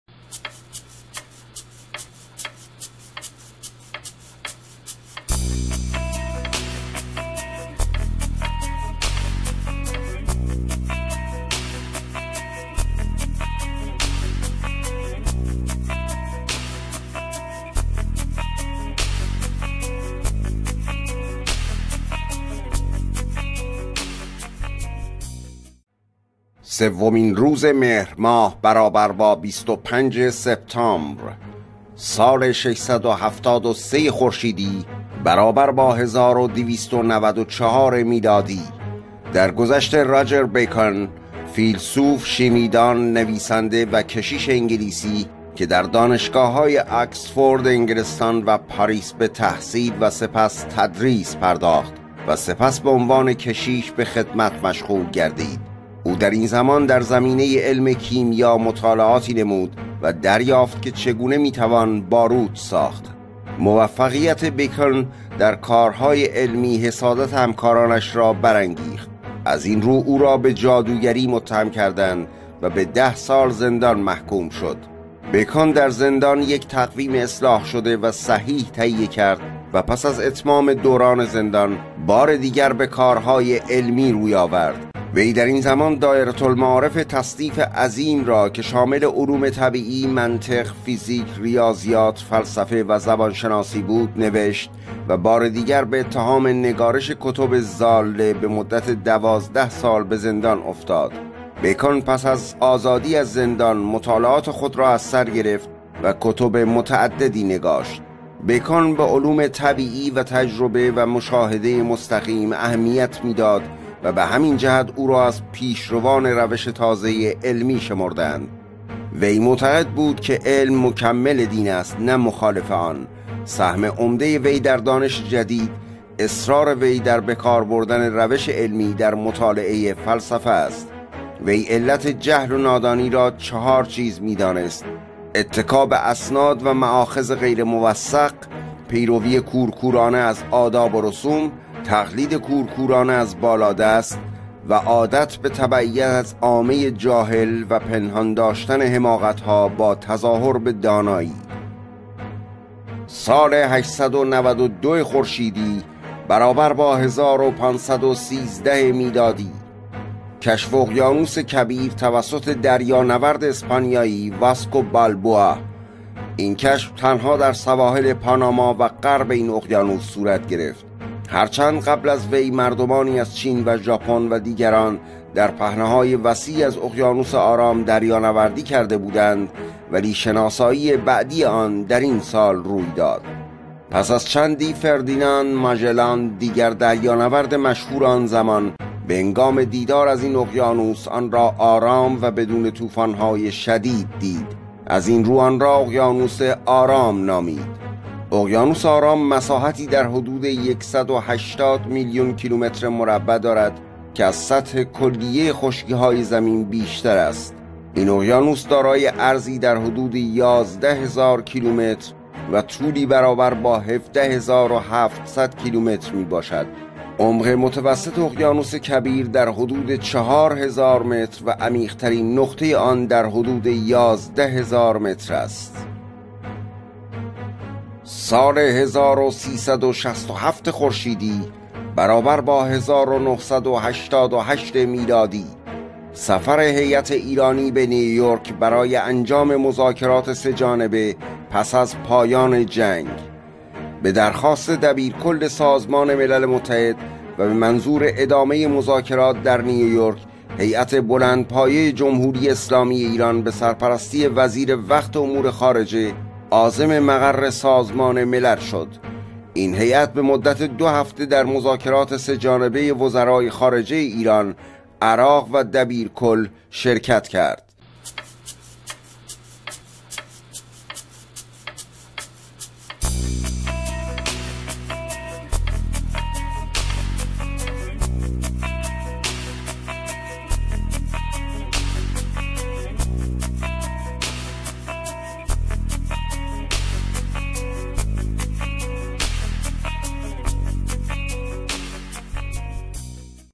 تلویزیون ایران فردا - تقویم تاریخ پنجشنبه 3 مهر
تقویم تاریخ را ورق بزنید تا از آنچه سالیان پیش در روز 3 مهر ماه به وقوع پیوست مطلع شوید. با ما همراه باشید.